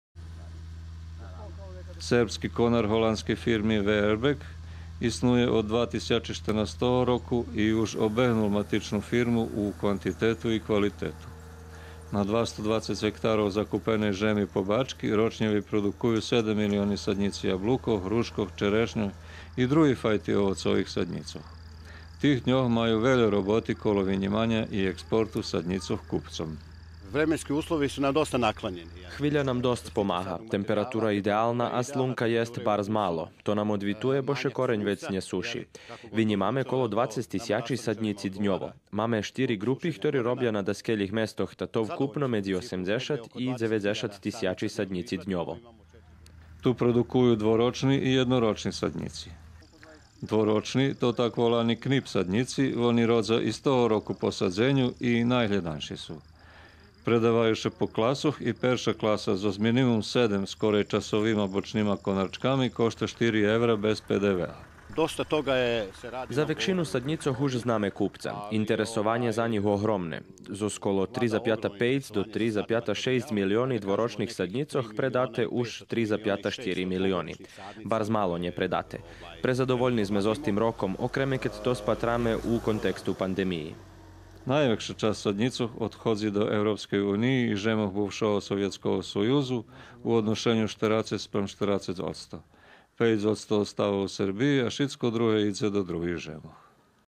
Продукция саднїцох у керестурским хотаре/Sadnjici
У хотаре Керестура єдна кулска фирма на даскелїх местох закупела жем и продукує саднїци овоцох . Наша екипа прешлого тижня зазначела винїманє тих саднїцох за предай.